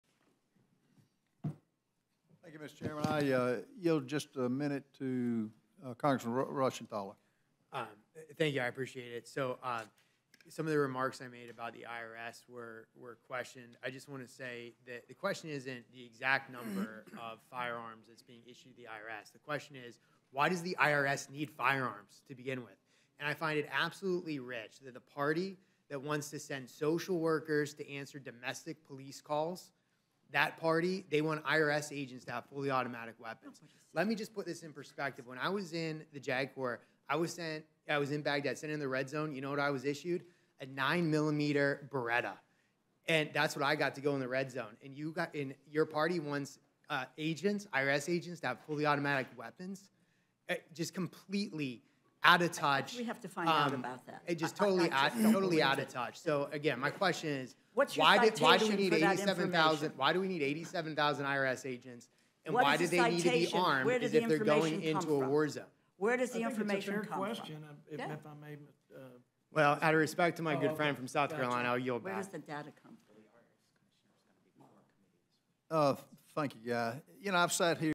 Guy Reschenthaler Asks Rosa DeLauro: 'Why Does The IRS Need Firearms?'
At last night's House Rules Committee hearing, Rep. Guy Reschenthaler (R-PA) questioned Rep. Rosa DeLauro (D-CT) about IRS agents.